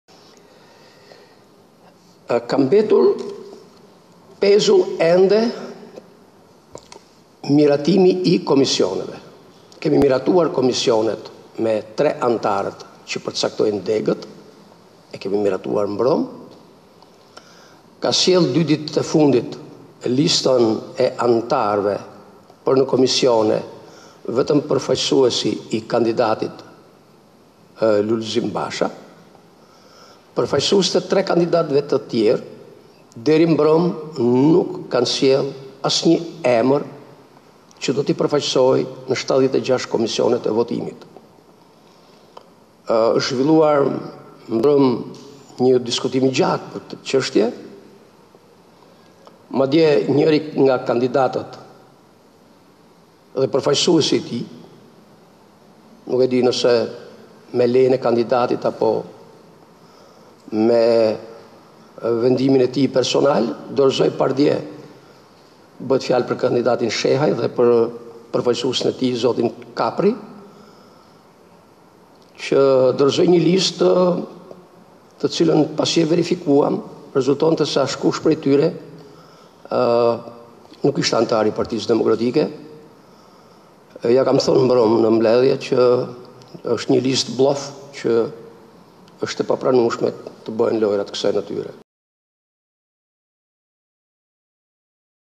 Gara për kreun e PD, Gjana: 3 kandidatë s’kanë sjellë emrat e anëtarëve të Komisionit të Votimit
Tiranë   Gara për kreun e PD, Gjana: 3 kandidatë s’kanë sjellë emrat e anëtarëve të Komisionit të Votimit